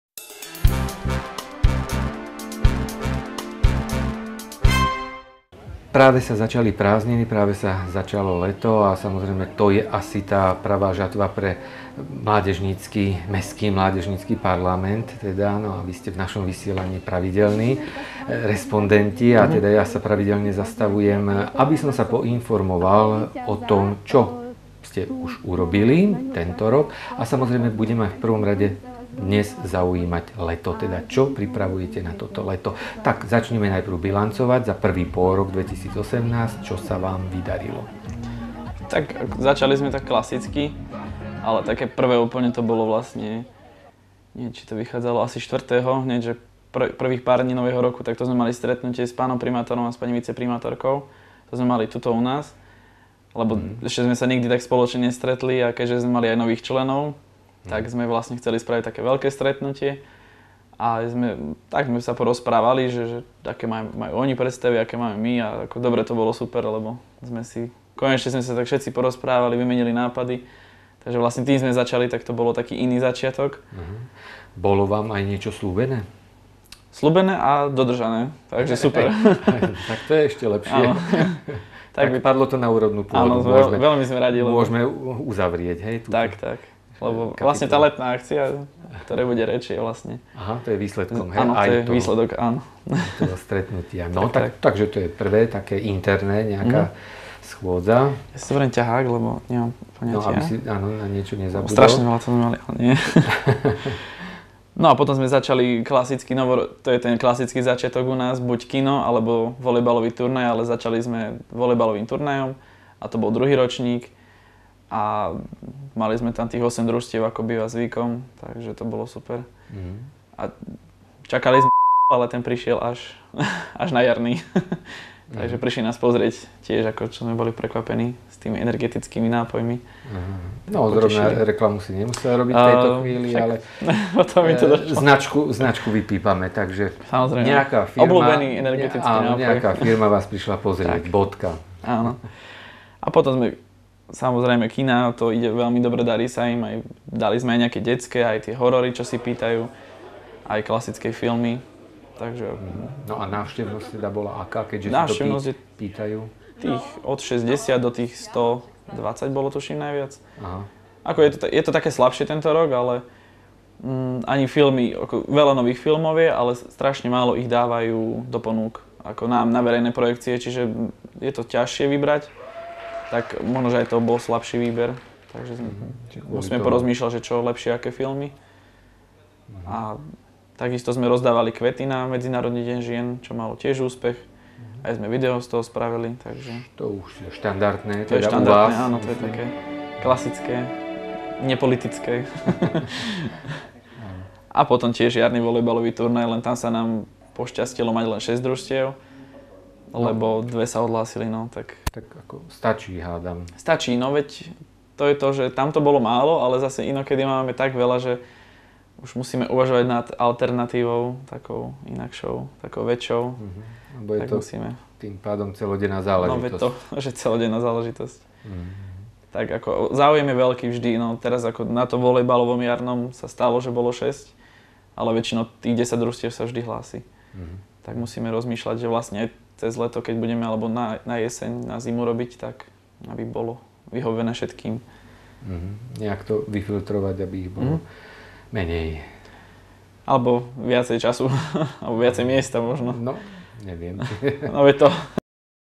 Leto v MMP
Rozhovor s predsedom MMP v Žarnovici o pripravovaných akciách.